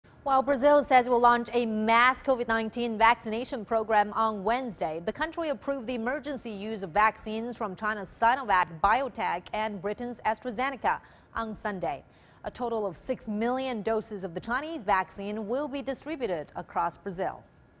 CORONAVIRUS PANDEMIC
Brazil says it will launch a mass COVID-19 vaccination program on Wednesday.
The country approved the emergency use of vaccines from China’s Sinovac Biotech and Britain’s AstraZeneca on Sunday.
A total of 6 million doses of the Chinese vaccine will be distributed across Brazil.